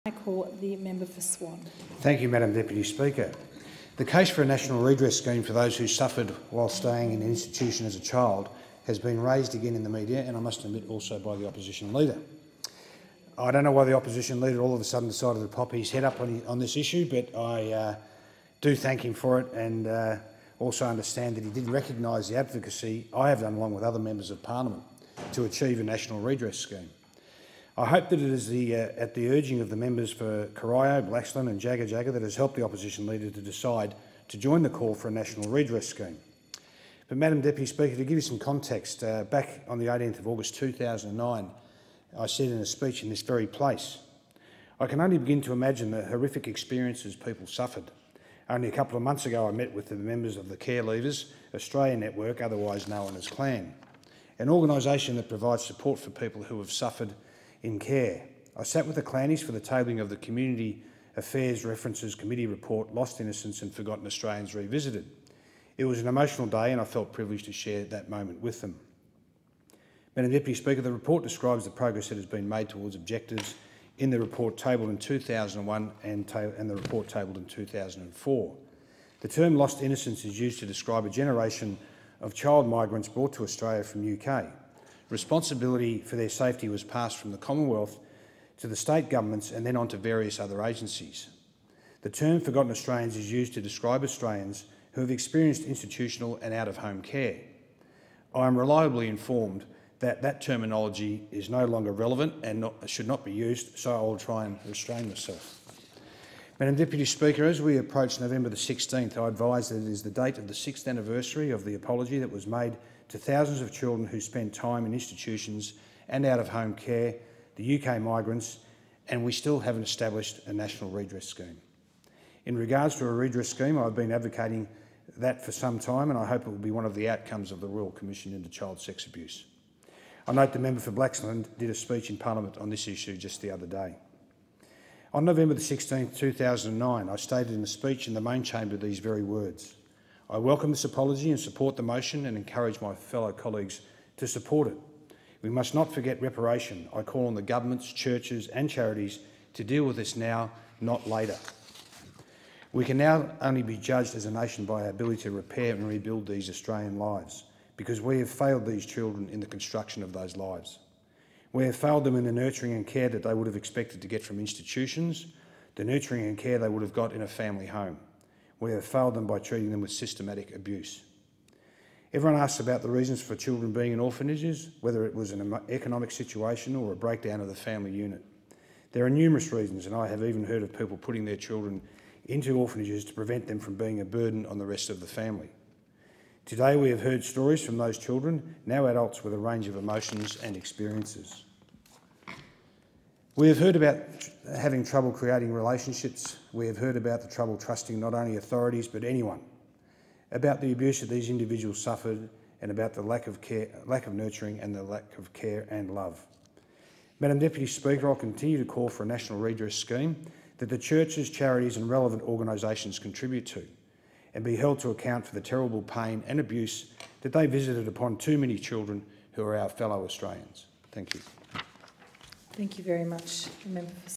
Royal Commission into Institutional Responses to Child Sexual Abuse
Steve Irons reiterates his call for a national redress scheme that the churches, charities and relevant organisations contribute to and for them to be held to account for the pain and abuse that they have visited upon too many children.